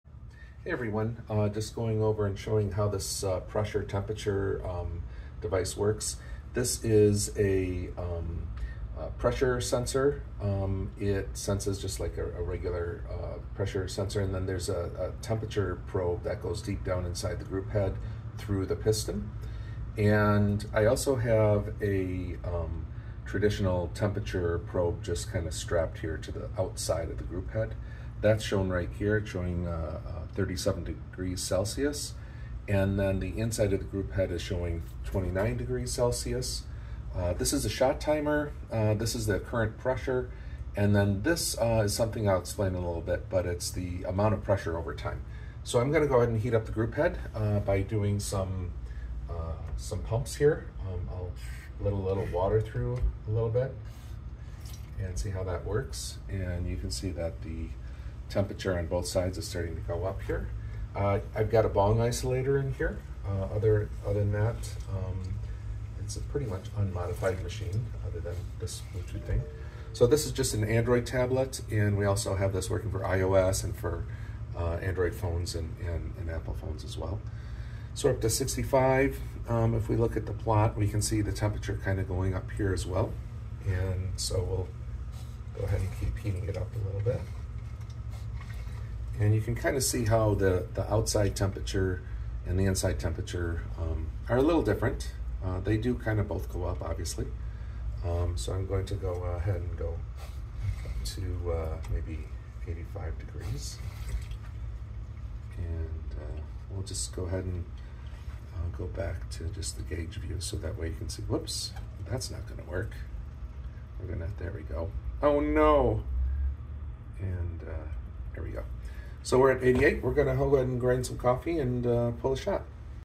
1. La Pavoni Pressure & Temperature Profiling - Heating Up
This is a demonstration of a prototype Bluetooth pressure and temperature sensor and app for the La Pavoni lever espresso machines.